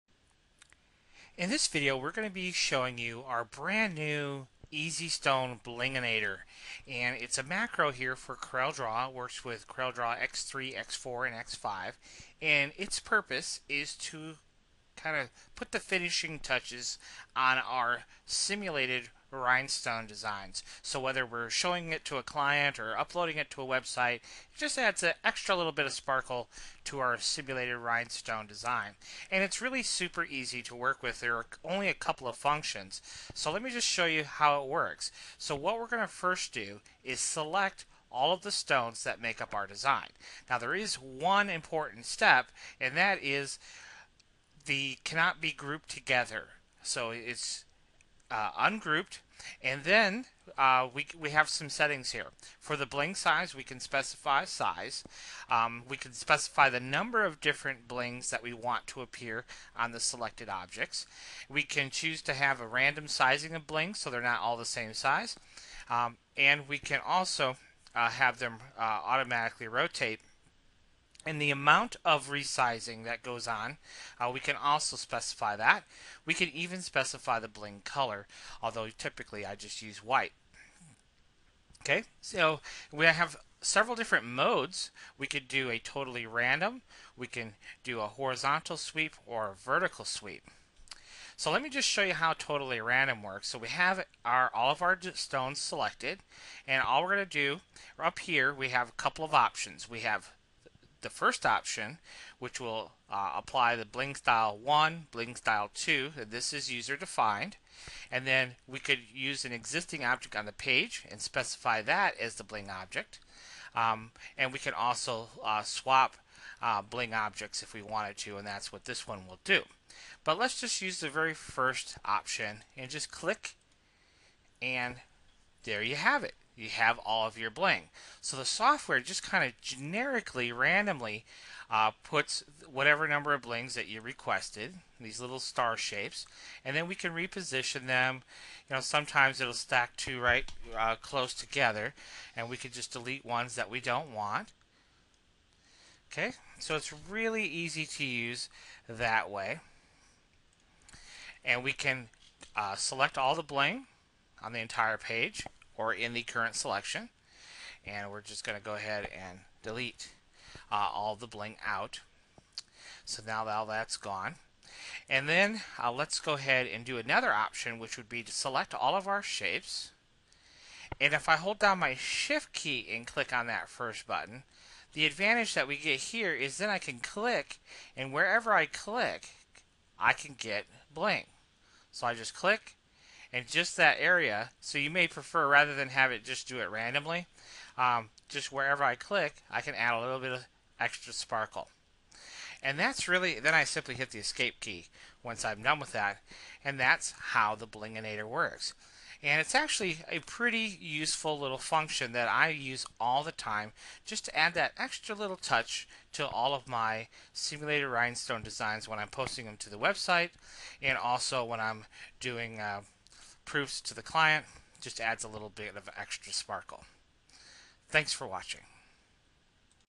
Introducing the Easy Stone - Rhinestone Bling-inator Macro for CorelDRAW
IN this video we show you our new Easy Stone Bling-inator Macro for CorelDRAW. This macro will help you add that little extra touch to your simulated Rhinestone Designs.

The makers of EasyStone - CorelDRAW Macro